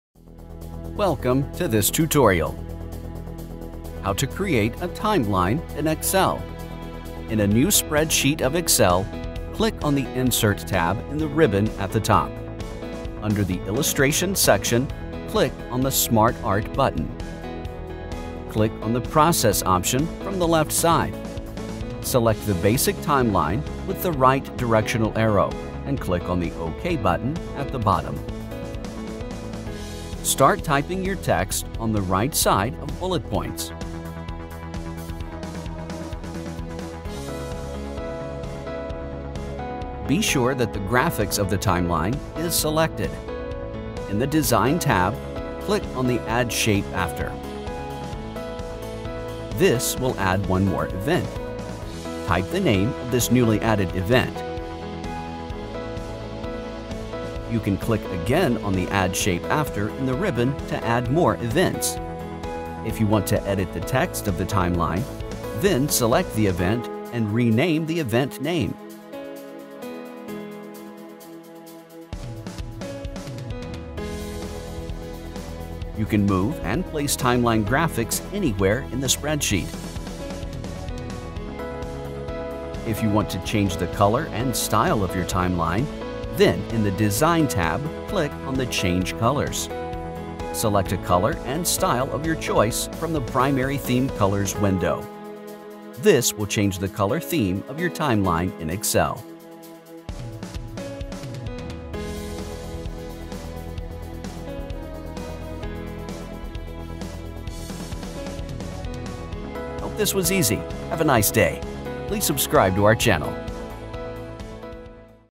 How to Create a Timeline in Excel
If you want to create a timeline in Excel, then it is possible in the 2013 or later versions of Excel. Let’s see the steps to create a timeline in Excel.

1. In a new spreadsheet of Excel, click on the Insert tab in the Ribbon at the top.
2. Under the illustrations section, click on the SmartArt button.
3. Click on the Process option from the left side.
4. Select the Basic Timeline with the right directional arrow, and click on the OK button at the bottom.
5. Start typing your text on the right side of bullet points.
6. Be sure that the graphics of the timeline is selected.
7. In the Design tab, click on the Add Shape After.
8. This will add one more event.
9. Type the name of this newly added event.
10. You can click again on the Add Shape After in the ribbon to add more events.
11. If you want to edit the text of the timeline, then select the event and rename the event name.
12. You can move and place timeline graphics anywhere in your spreadsheet.
13. If you want to change the color and style of your timeline, then in the Design tab, click on the Change Colors.
14. Select a color and style of your choice from the Primary Theme Colors window.
15. This will change the color theme of your timeline in Excel.

Hope this was easy. Have a nice day!